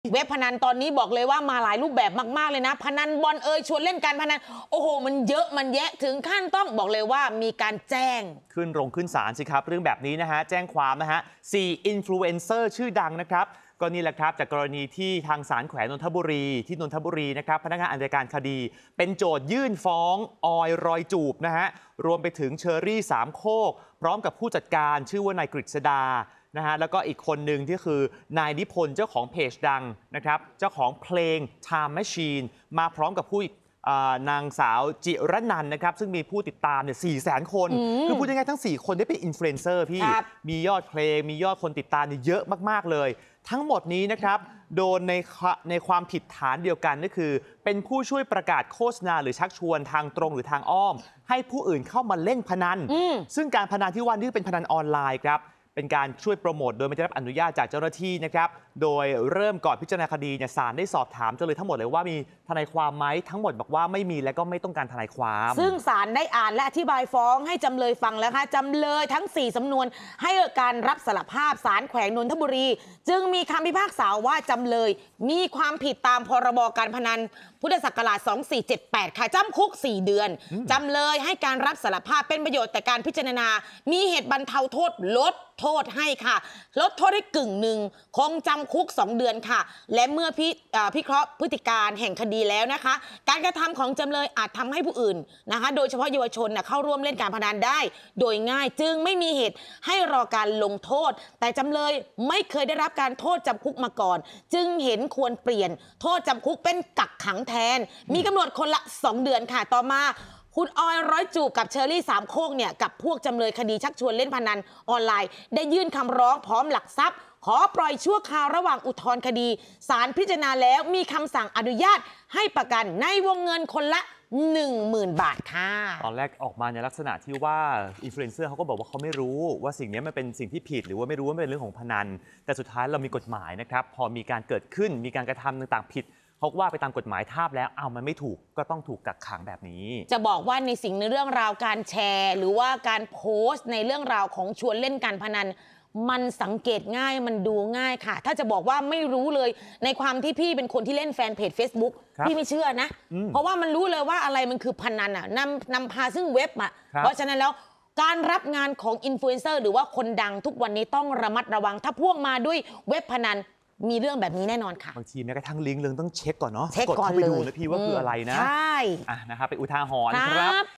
ศาลฯสั่ง กักขัง 2 เดือนแทนจำคุก "4 อินฟลูเอนเซอร์" ปมโฆษณาเว็บพนันออนไลน์ I 18-07-66
ศาลฯสั่ง กักขัง 2 เดือนแทนจำคุก "4 อินฟลูเอนเซอร์" ปมโฆษณาเว็บพนันออนไลน์